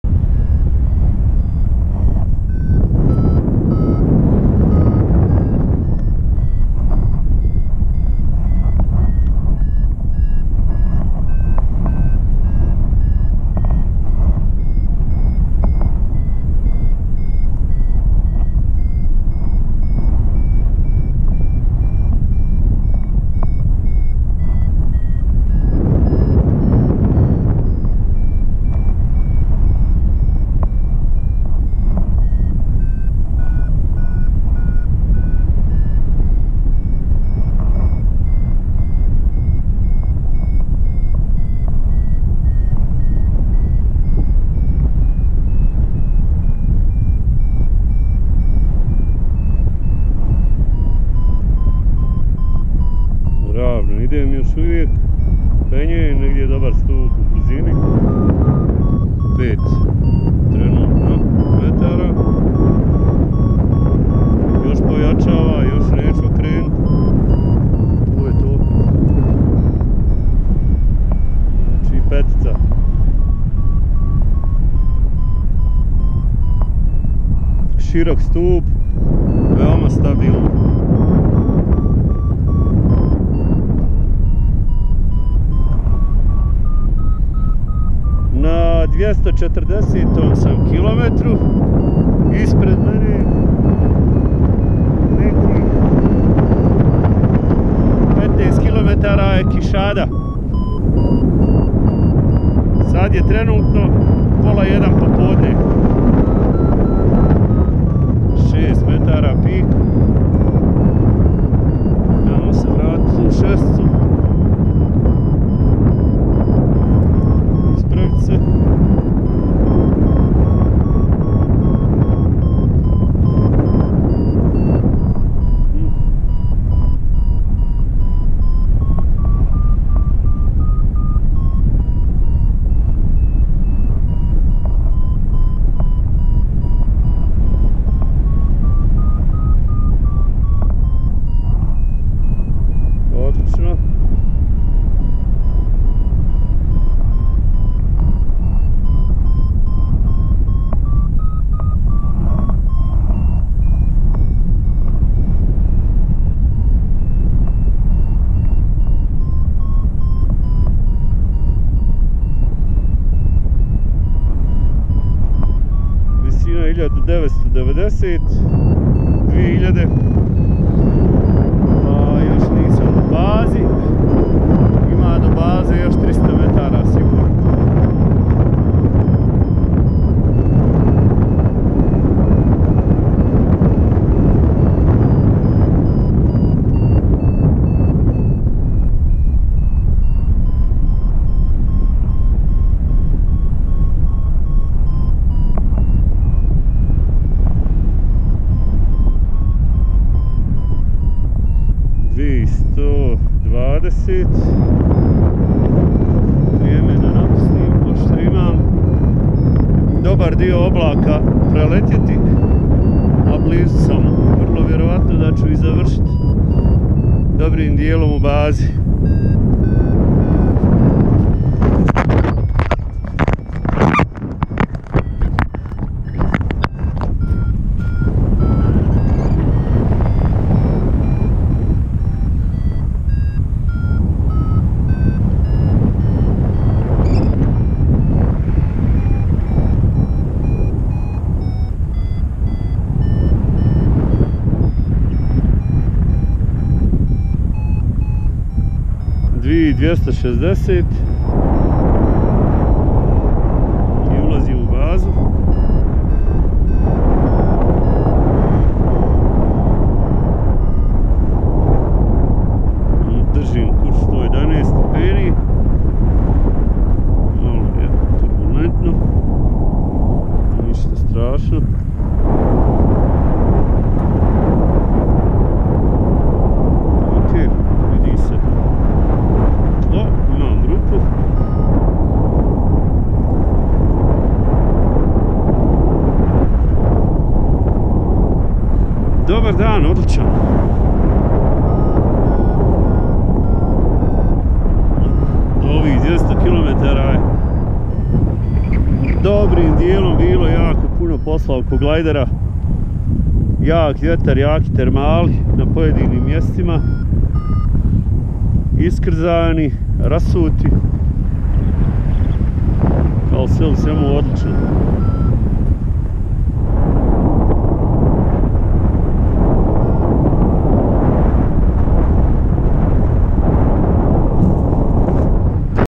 Riding the Brazilian Skies: Quixadá's Wide&Smooth Thermal +5m/s
Join me on an unforgettable thermal climb over Quixada, Brazil. It was on my route where I cover a distance of 404.8 kilometers – a new personal best for me! This exhilarating flight took me through captivating landscapes, including a breathtaking thermal lift above a field that had wildfire. Against all expectations, the thermal updraft remained surprisingly stable, despite the fiery conditions below. While this entire flight presented its challenges in terms of paraglider control in turbulence, riding this particular thermal column was an absolute delight. I ascended steadily and smoothly, with an expansive view of Quixada, the city and take odd I found myself above. This memorable moment occurred at approximately the 240-kilometer mark of my journey." Enjoy the video and the mesmerizing views!